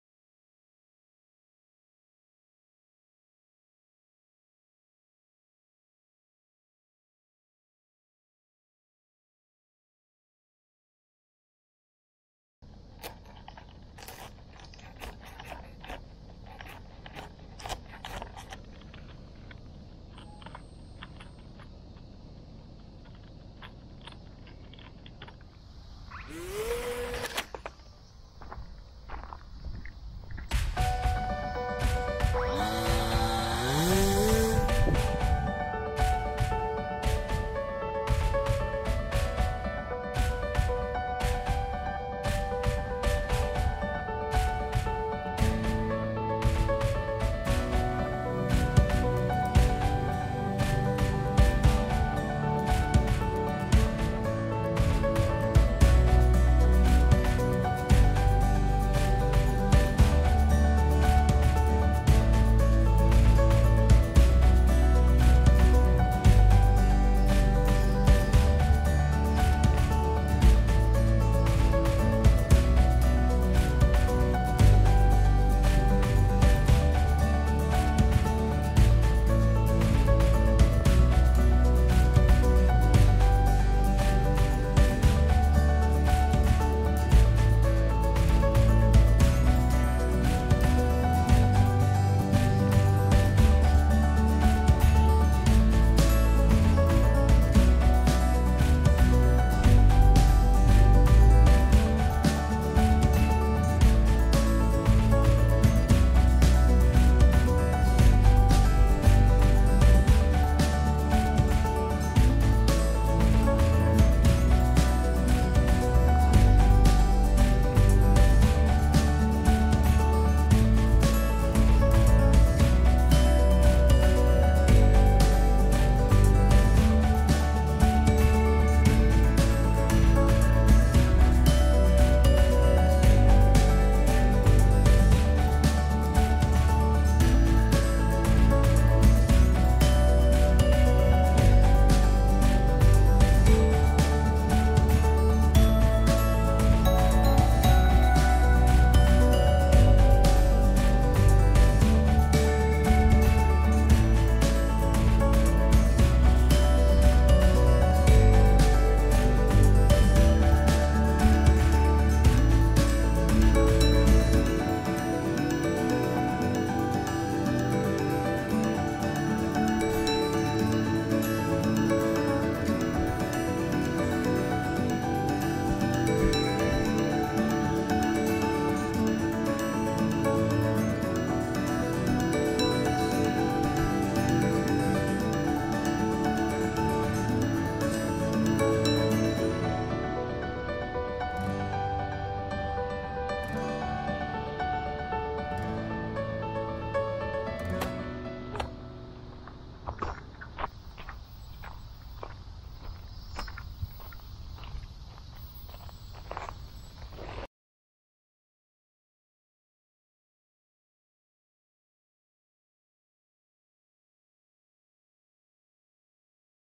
XK A160 ラジコン飛行機
プロぺラはA600・A430と同じです。
楽に飛ばせる機体ですが、最近「超」楽に飛ばせる機体ばかりでしたので落としました。